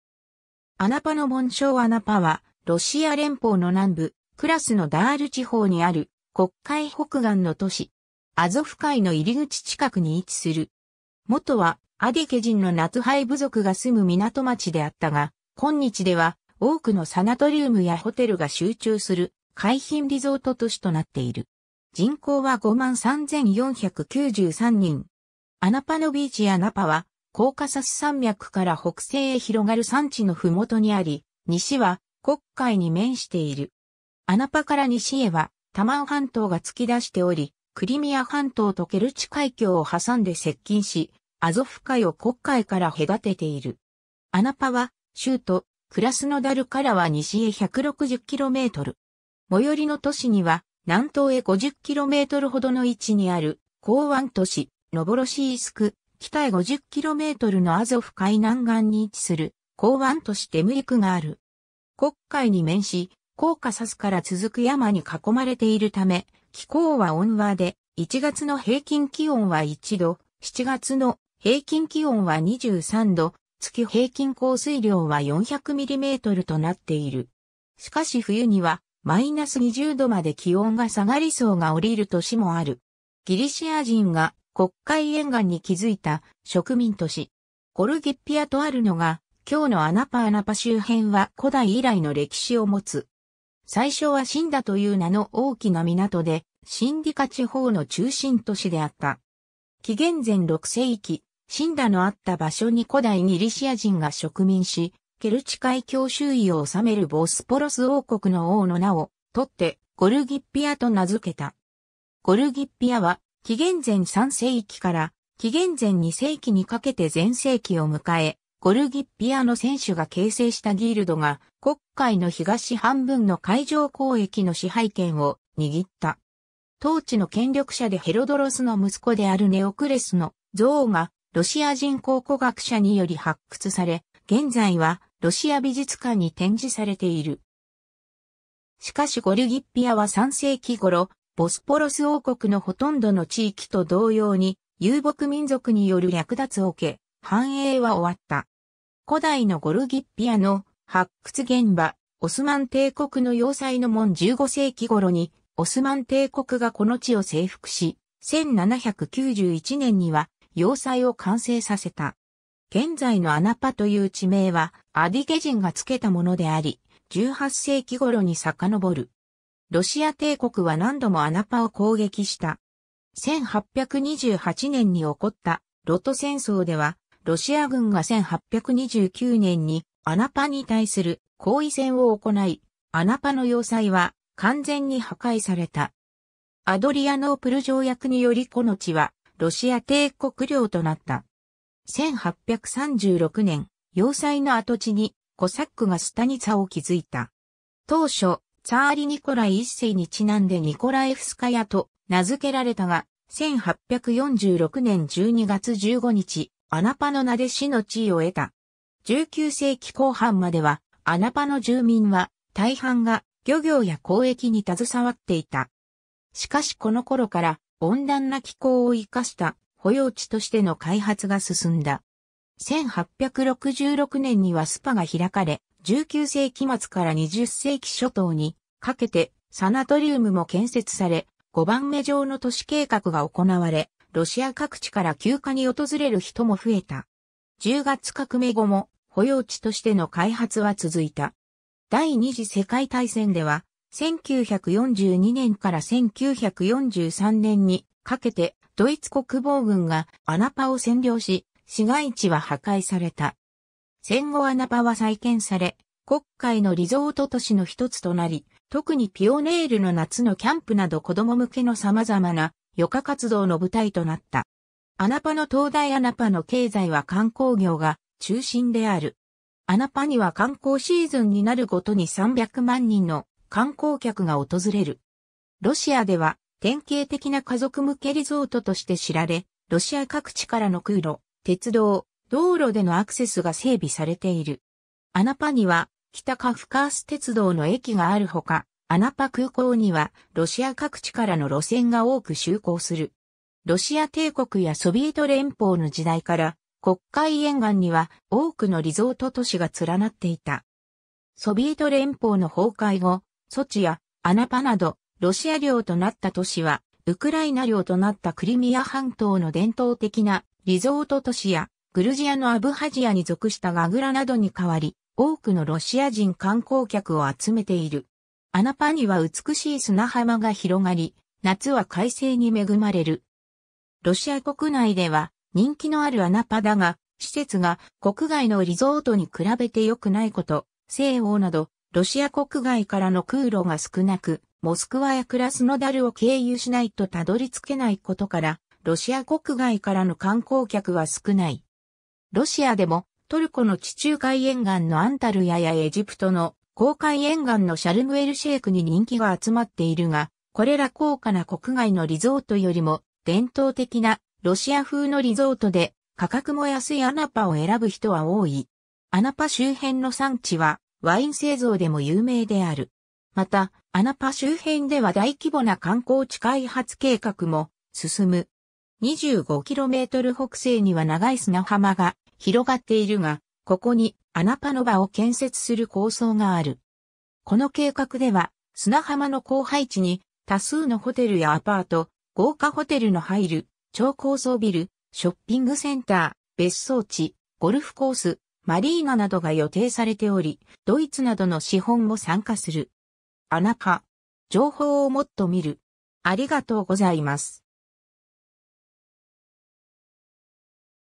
アナパ
アナパの紋章 アナパ（アナーパ、ロシア語: Ана́па、アディゲ語: Быгъуркъал、Anapa）はロシア連邦の南部、クラスノダール地方にある黒海北岸の都市。
アゾフ海の入口近くに位置する。
もとはアディゲ人のナトゥハイ部族が住む港町であったが、今日では多くのサナトリウムやホテルが集中する海浜リゾート都市となっている。
 アナパのビーチ アナパはコーカサス山脈から北西へ広がる山地のふもとにあり、西は黒海に面している。
アナパから西へはタマン半島が突き出しており、クリミア半島とケルチ海峡を挟んで接近し、アゾフ海を黒海から隔てている。
 アナパは州都クラスノダルからは西へ160km。
最寄の都市には、南東へ50kmほどの位置にある港湾都市ノヴォロシースク、北へ50kmのアゾフ海南岸に位置する港湾都市テムリュクがある。
 黒海に面しコーカサスから続く山に囲まれているため、気候は温和で、1月の平均気温は1度、7月の平均気温は23度、月平均降水量は400mmとなっている。
しかし冬にはマイナス20度まで気温が下がり霜が下りる年もある。
 ギリシア人が黒海沿岸に築いた植民都市。
ゴルギッピア（Gorgippia）とあるのが今日のアナパ アナパ周辺は古代以来の歴史をもつ。
最初はシンダ（Sinda）という名の大きな港で、シンディカ（Sindica）地方の中心都市であった。
紀元前6世紀、シンダのあった場所に古代ギリシア人（ポントス人）が植民し、ケルチ海峡周囲を治めるボスポロス王国の王の名を採ってゴルギッピアと名づけた。
 ゴルギッピアは紀元前3世紀から紀元前2世紀にかけて全盛期を迎え、ゴルギッピアの船主が形成したギルドが黒海の東半分の海上交易の支配権を握った。
当地の権力者でヘロドロス（Herodoros）の息子であるネオクレス（Neokles）の像がロシア人考古学者により発掘され、現在はロシア美術館に展示されている。
しかしゴルギッピアは3世紀頃、ボスポロス王国のほとんどの地域と同様に遊牧民族による略奪を受け、繁栄は終わった。
 古代のゴルギッピアの発掘現場 オスマン帝国の要塞の門（1787年） 15世紀頃にオスマン帝国がこの地を征服し、1791年には要塞を完成させた。
現在のアナパという地名はアディゲ人がつけたものであり、18世紀頃に遡る。
 ロシア帝国は何度もアナパを攻撃した。
1828年に起こった露土戦争では、ロシア軍が1829年にアナパに対する攻囲戦を行い、アナパの要塞は完全に破壊された。
アドリアノープル条約によりこの地はロシア帝国領となった。
 1836年、要塞の跡地にコサックがスタニツァ（コサックの居住する村）を築いた。
当初、ツァーリ・ニコライ1世にちなんでニコラエフスカヤと名づけられたが、1846年12月15日、アナパの名で市の地位を得た。
 19世紀後半までは、アナパの住民は大半が漁業や交易に携わっていた。
しかしこの頃から温暖な気候を生かした保養地としての開発が進んだ。
1866年にはスパ（療養温泉）が開かれ、19世紀末から20世紀初頭にかけてサナトリウムも建設され、碁盤目状の都市計画が行われ、ロシア各地から休暇に訪れる人も増えた。
十月革命後も保養地としての開発は続いた。
 第二次世界大戦では、1942年から1943年にかけてドイツ国防軍がアナパを占領し、市街地は破壊された。
戦後アナパは再建され黒海のリゾート都市のひとつとなり、特にピオネールの夏のキャンプなど子供向けの様々な余暇活動の舞台となった。
 アナパの灯台 アナパの経済は観光業が中心である。
アナパには観光シーズンになるごとに300万人の観光客が訪れる。
ロシアでは典型的な家族向けリゾートとして知られ、ロシア各地からの空路、鉄道、道路でのアクセスが整備されている。
アナパには北カフカース鉄道の駅があるほか、アナパ空港にはロシア各地からの路線が多く就航する。
 ロシア帝国やソビエト連邦の時代から黒海沿岸には多くのリゾート都市が連なっていた。
ソビエト連邦の崩壊後、ソチやアナパなどロシア領となった都市は、ウクライナ領となったクリミア半島の伝統的なリゾート都市や、グルジアのアブハジアに属したガグラなどに代わり、多くのロシア人観光客を集めている。
 アナパには美しい砂浜が広がり、夏は快晴に恵まれる。
ロシア国内では人気のあるアナパだが、施設が国外のリゾー...